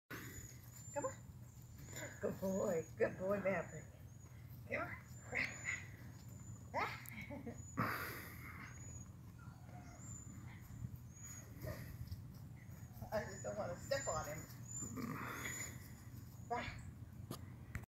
Mavericks 1st day
this is Maverick, purebred german shepherd, picked him.out of the litter at 2 weeks old, he is 6 weeks old today, just brought him home, this is his first time in the back yard,